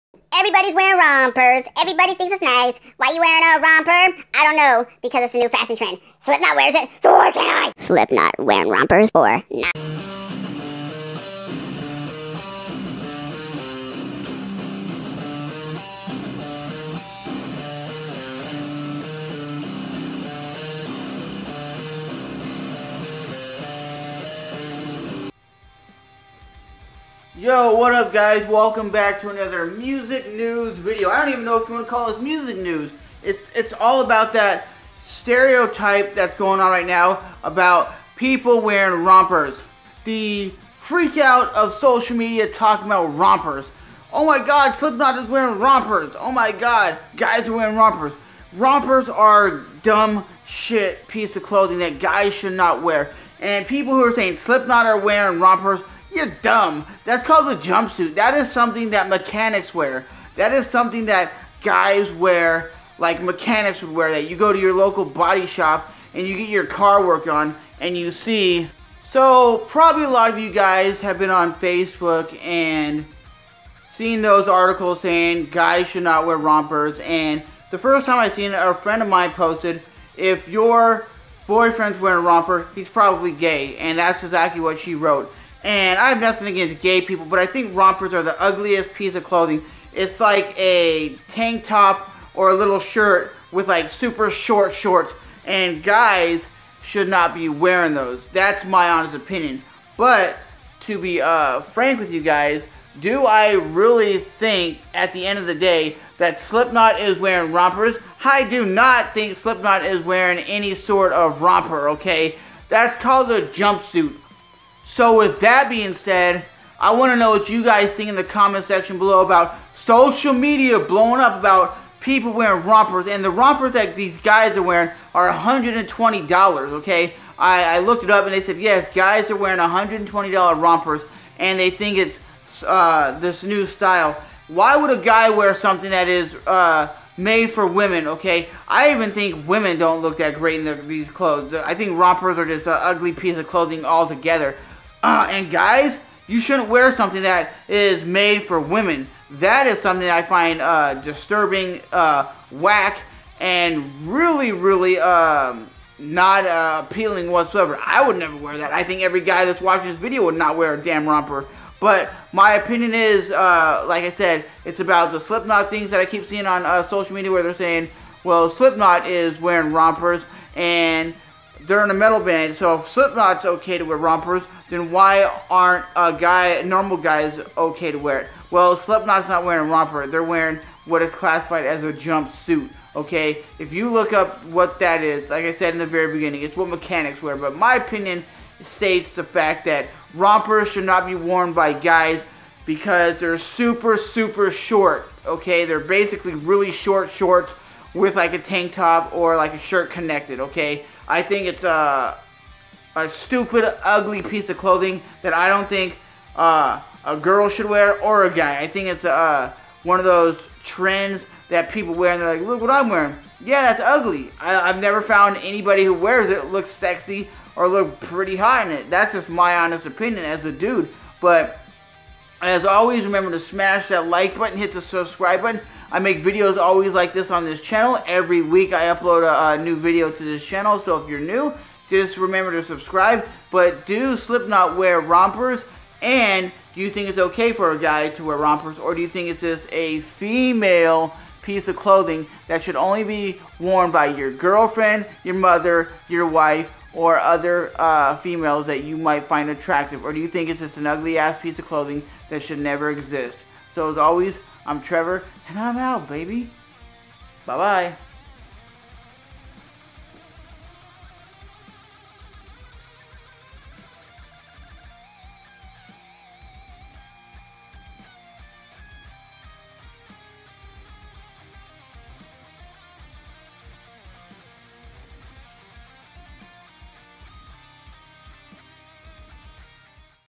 does Slipknot wear rompers MUSIC NEWS
i don't get it but i'm talking about
slipknot wearing rompers or is it ajump suit

S O C I A L